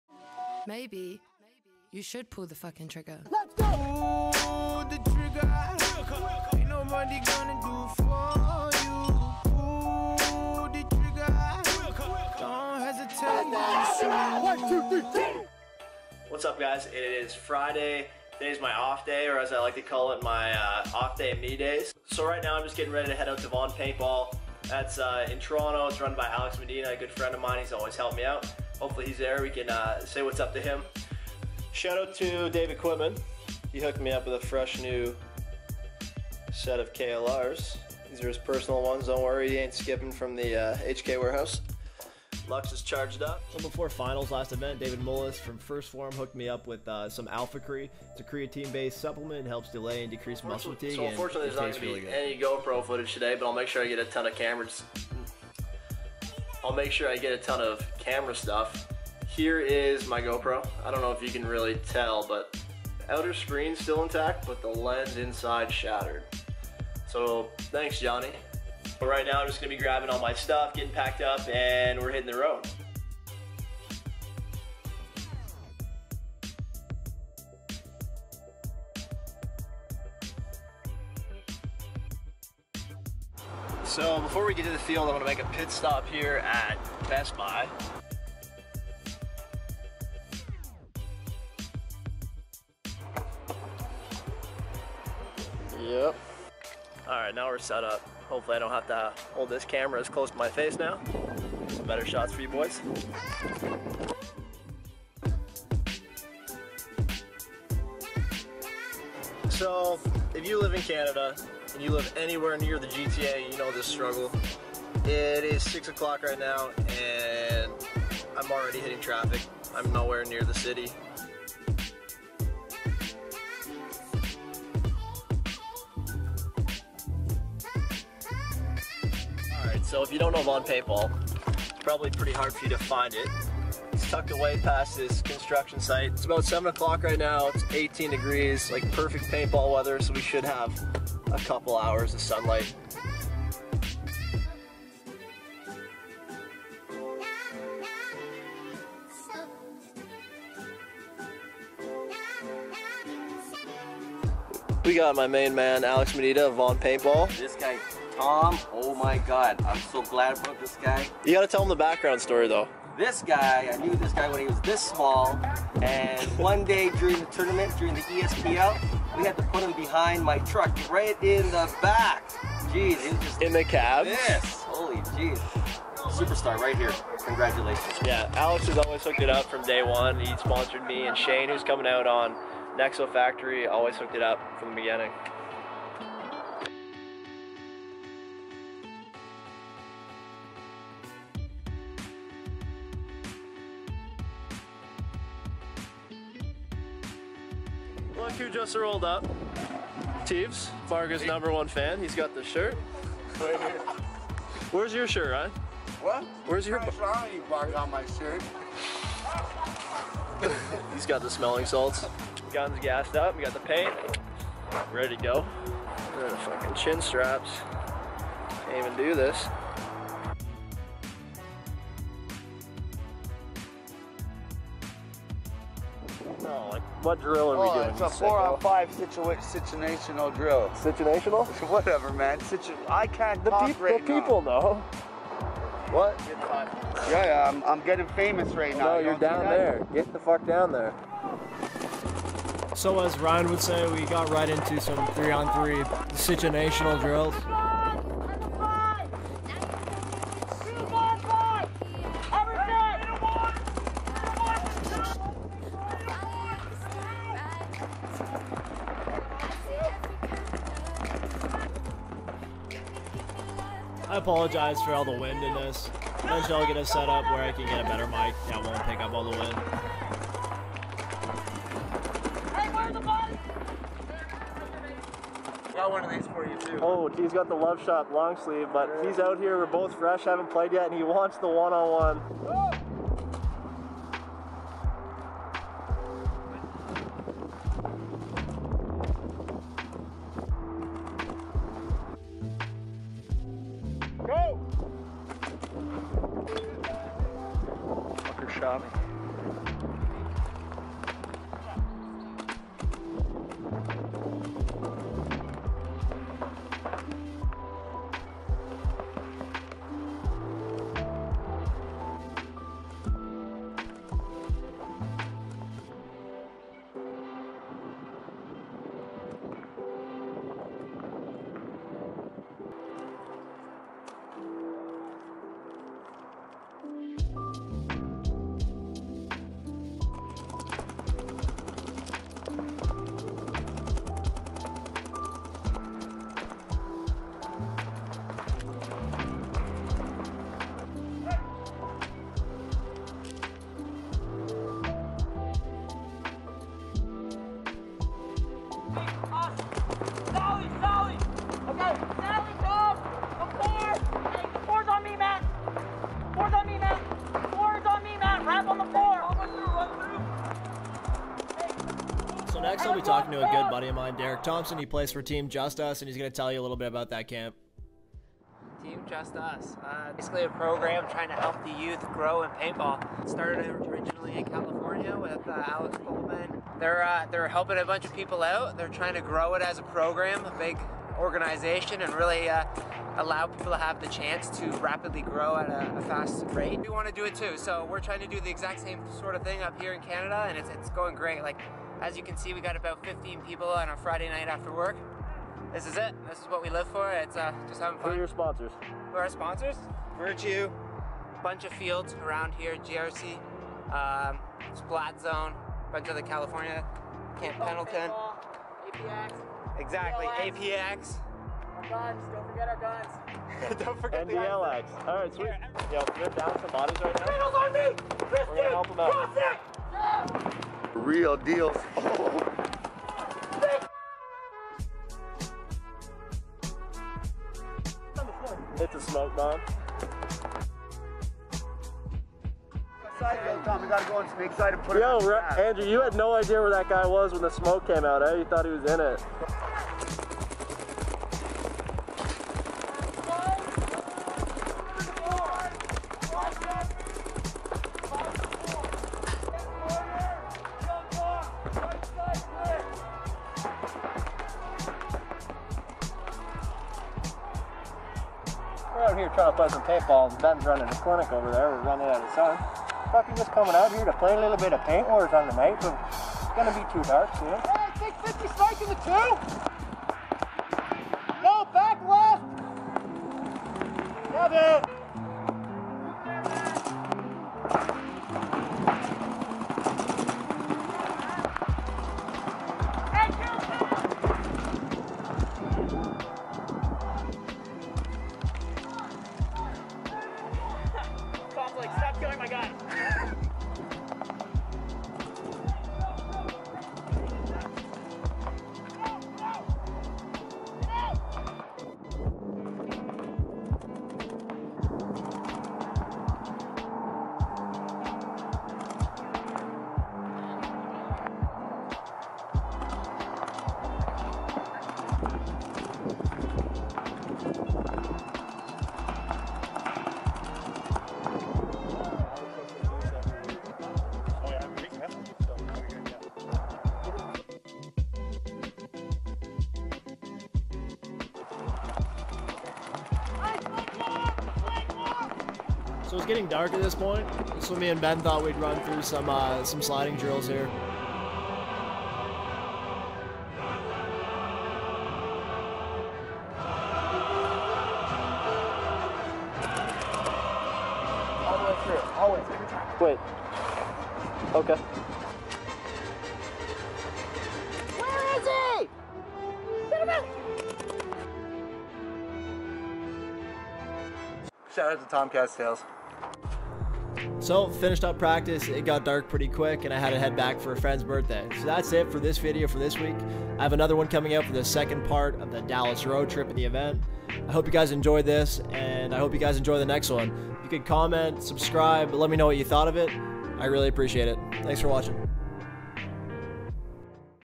WE GOT SMOKE BOMBS... // Paintball Vlog
Friday night paintball vlog in Toronto, we used smoke bombs !